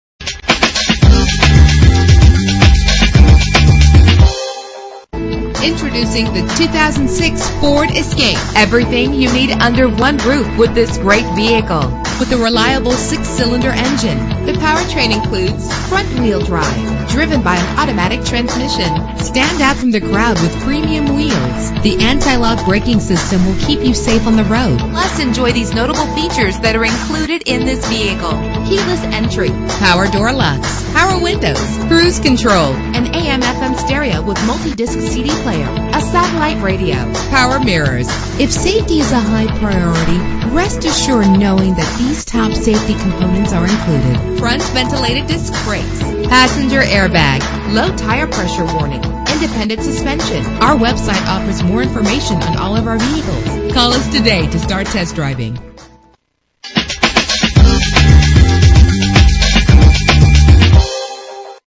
2006 Ford Escape - Little Rock AR
Year: 2006
Make: Ford
Model: Escape
Trim: XLT
Engine: 3.0 liter 6 cylinder 24 valve
Transmission: 4-Speed Automatic
Color: Orange
Mileage: 142217
Address:
4600 South University Ave